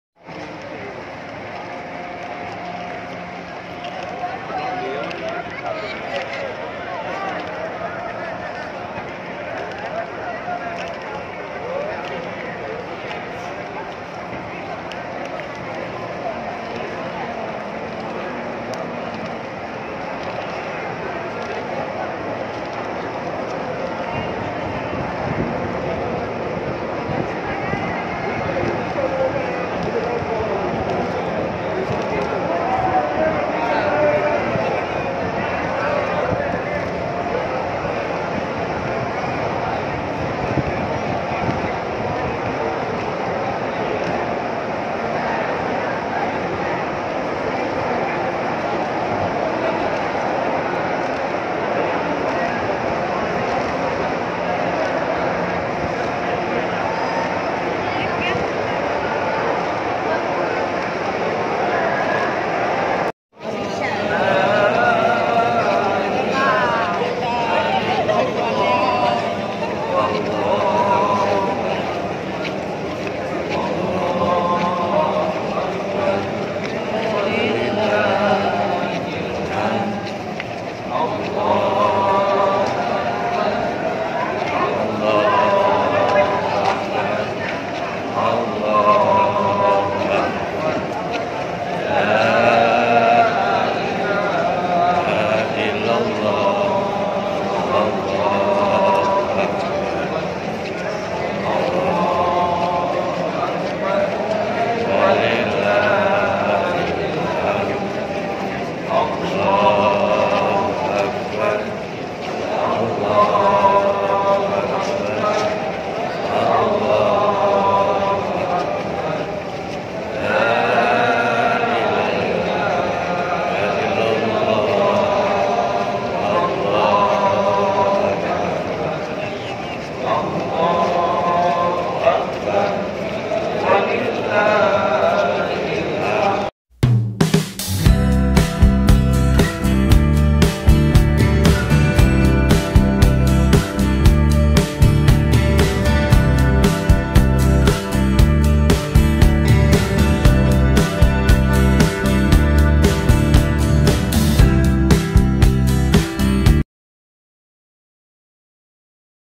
Terowongan Mina Dipadati Jamaah Asal Indonesia
Jemaah haji di Mina harus menempuh perjalanan panjang saat lempar jumrah ke jamarat. Dari tenda pemondokan ke mulut terowongan jaraknya bervariasi antara 500 meter hingga 1,5 km. Sementara panjang terowongan sekitar 2 km atau pulang pergi 4 km